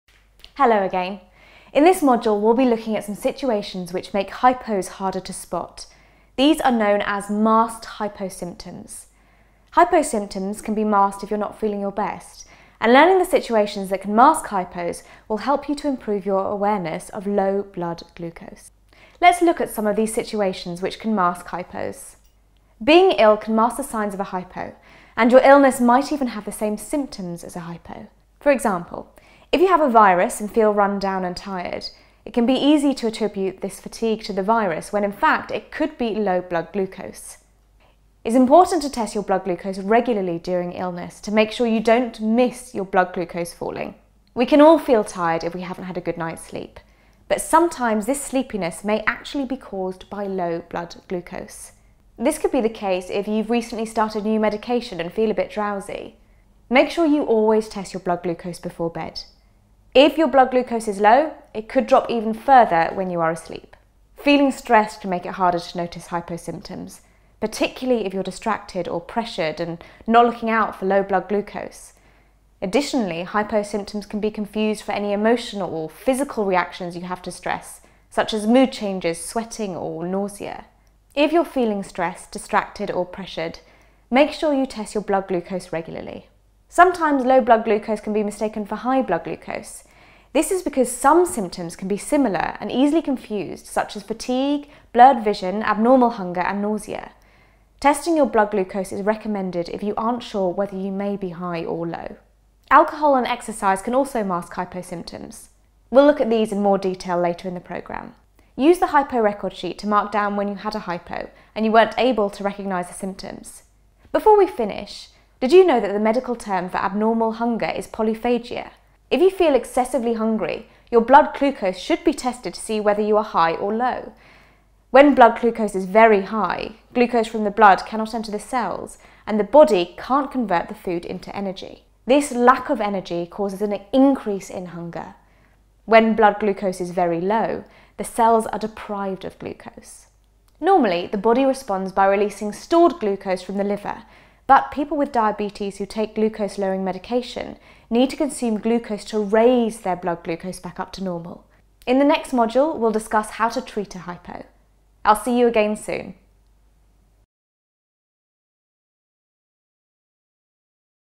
Hidden hypo signs and symptoms
Whilst many of us think of diabetes as being a problem of high blood sugar levels, the medication some people with diabetes take can also cause their sugar levels to go too low and this can become dangerous. Here's a video guide to masked signs and symptoms of a hypo.